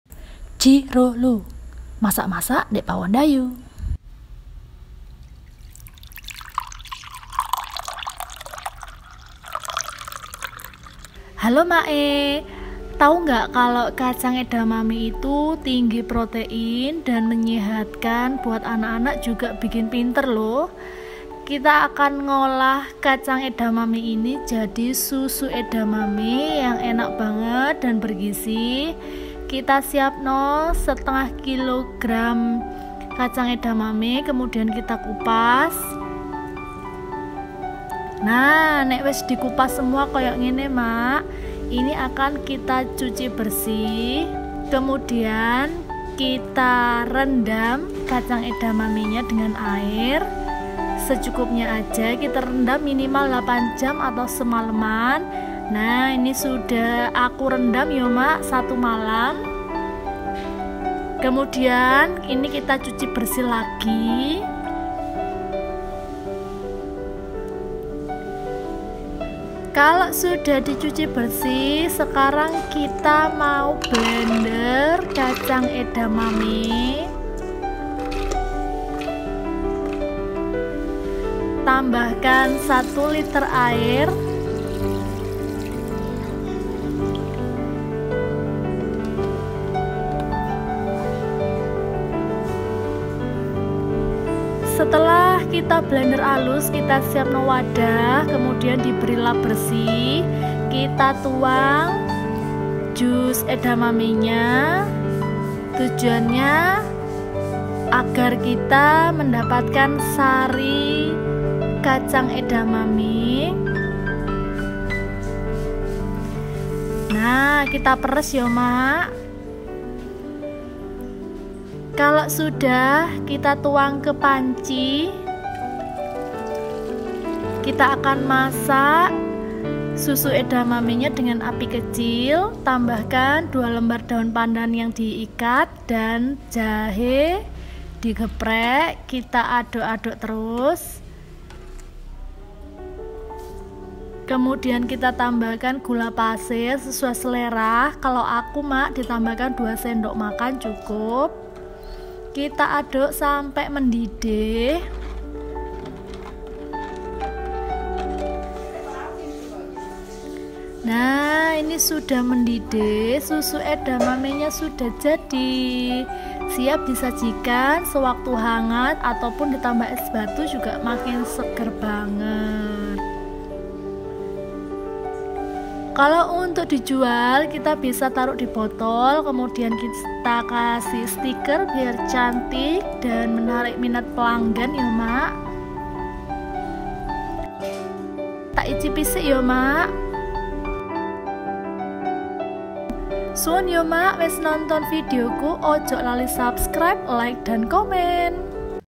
RESEP SUSU KACANG KEDELAI EDAMAME & MANFAATNYA BAGI KESEHATAN
Cara Membuat Susu Kacang Kedelai Edamame yang Enak, Bergizi, dan Sehat untuk Kesehatan & Ana-anak.

Halo Mak, di video kali ini aku mau berbagi resep cara mengolah kacang edamame menjadi susu edamame yang enak sekali dan menyehatkan. Berdasarkan sumber artikel alodokter bahwa di dalam 100 gr kedelai edamame terkandung sekitar 121 kalori dan beberapa nutrisi yaitu protein, karbohidrat, erat, zat besi, magnesium, kalsium,  zinc, juga mengandung vitamin A, vitamin C, vitamin E, vitamin K, dan antioksidan, seperti isoflavon.
Manfaat Bagi Kesehatan :
1. Menurunkan kadar kolesterol jahat
2. Mengurangi risiko terjadinya sejumlah penyakit
3. Meredakan gejala menopause
4. Menyehatkan saluran pencernaan
5. Menjaga berat badan tetap ideal
Tidak hanya itu, si Kecil juga bisa mengonsumsi jenis kacang yang satu ini karena mengandung berbagai nutrisi penting yaitu :
- Meningkatkan daya tahan tubuh
- Menjaga kesehatan tulang
- Menjaga kesehatan kulit
- Menjaga kesehatan sistem pencernaan
Sumber Artikel : ibudanbalita

Bahan Susu Edamame :
Edamame 500 gram
Air 1 L
Daun Pandan 2 lembar simpul ikat
Jahe 1 ruas geprek
Gula pasir 2-3 sdm